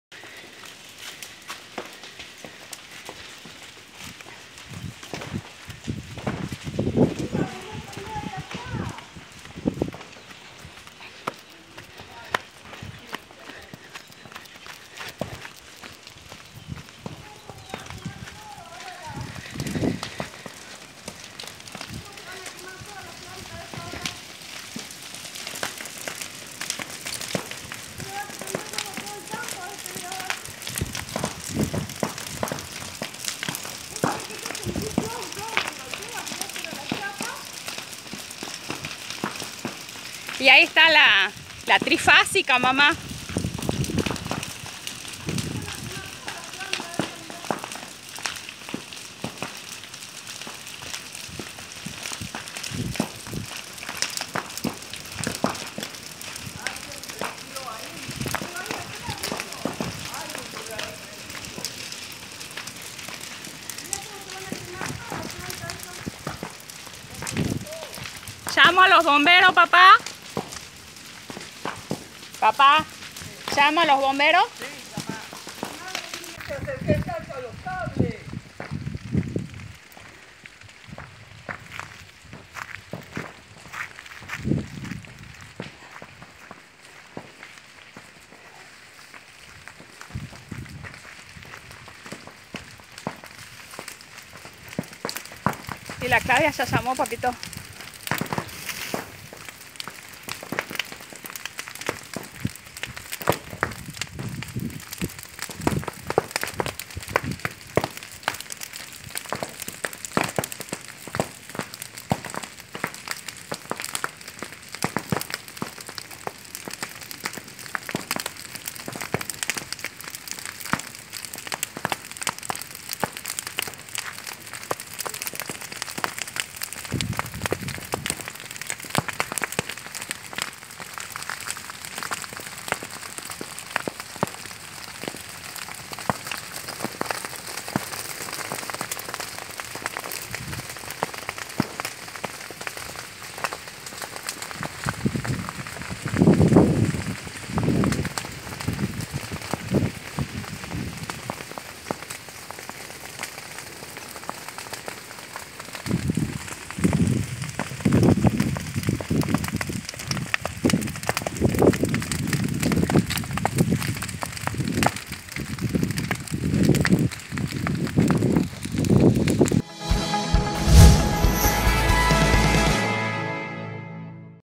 Brigadistas controlaron 13 incendios forestales este domingo en Jujuy
Se produjeron siniestros en Fraile Pintado, Ocloyas y hubo dos focos importantes en Sausalito. La mayoría fueron intencionales.